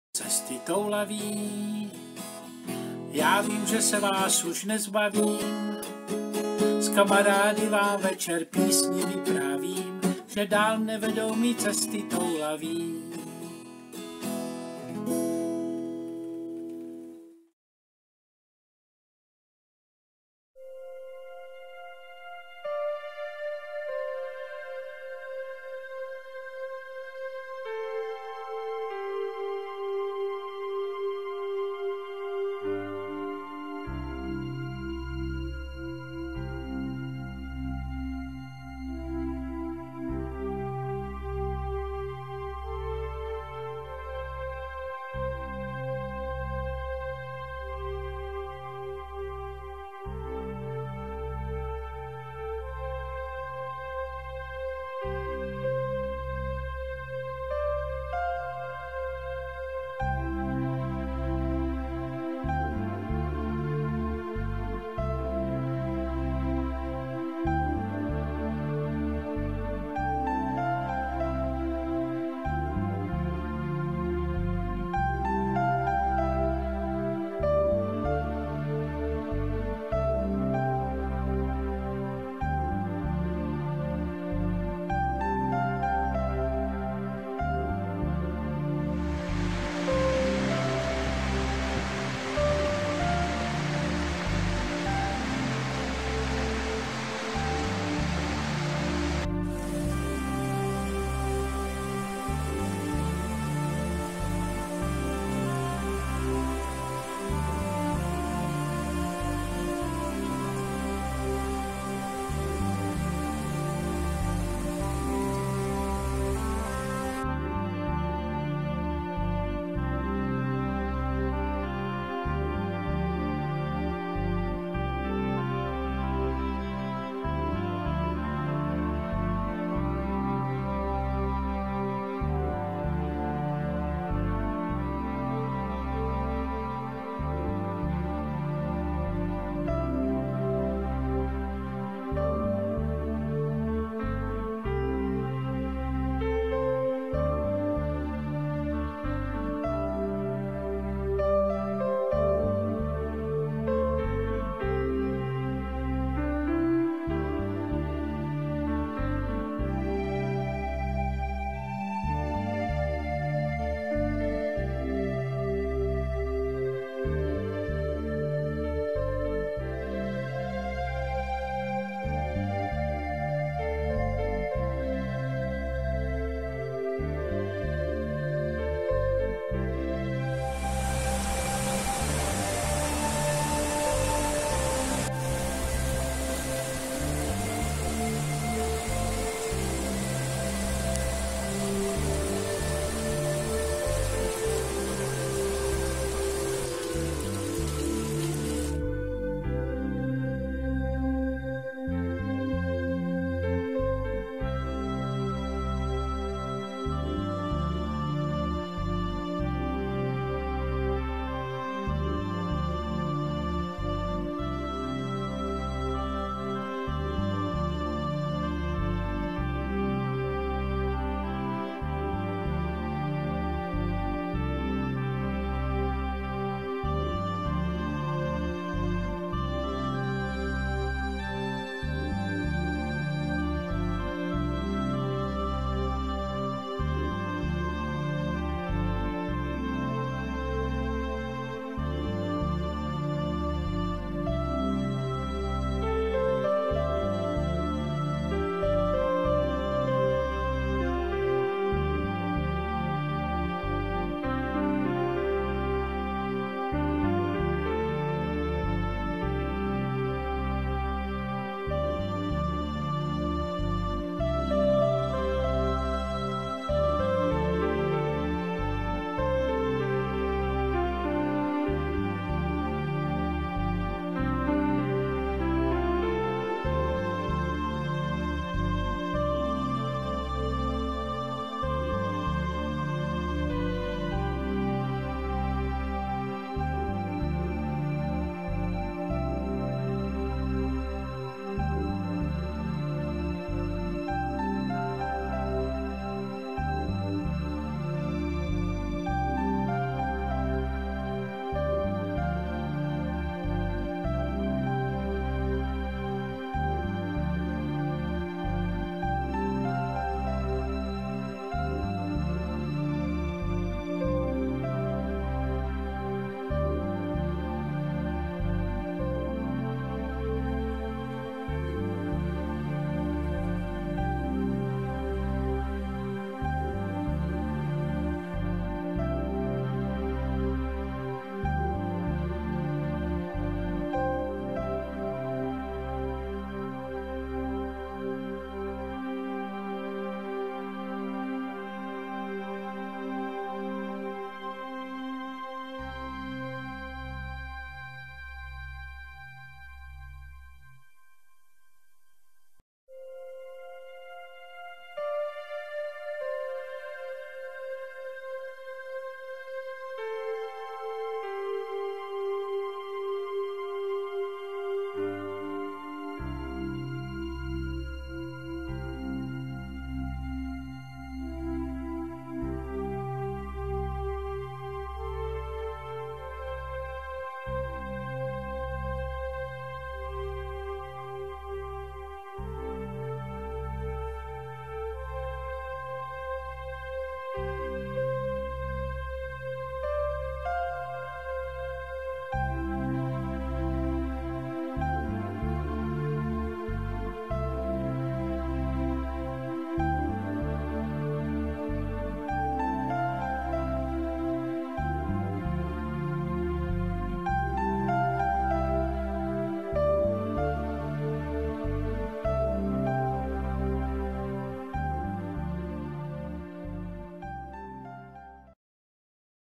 Šumavou Karla Klostermanna
V cyklu cesty toulavý. Srní, řeka Vydra, hrad Kašperk